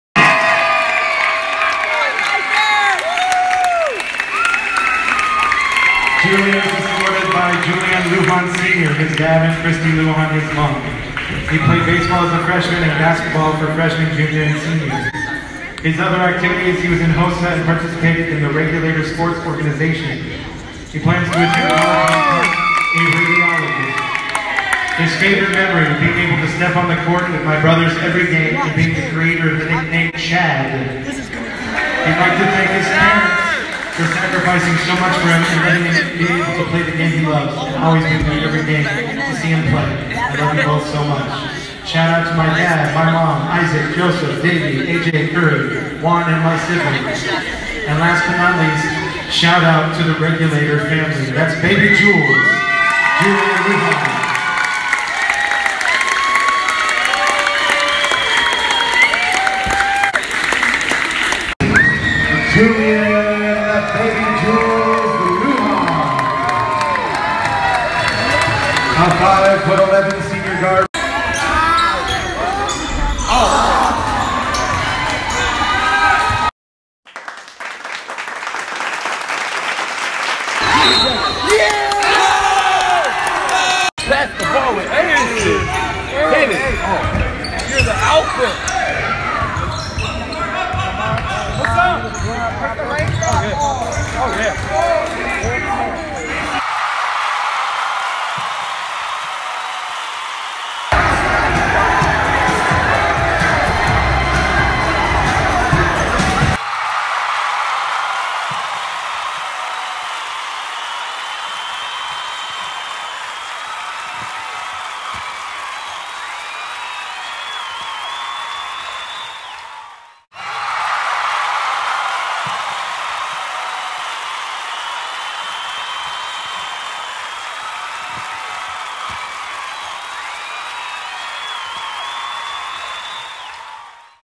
senior night “jules”
Honored to attend my God son’s senior night. He hustled and played an awesome game. He puts so much effort and time into basketball I am glad he was able to do such a great job in high school!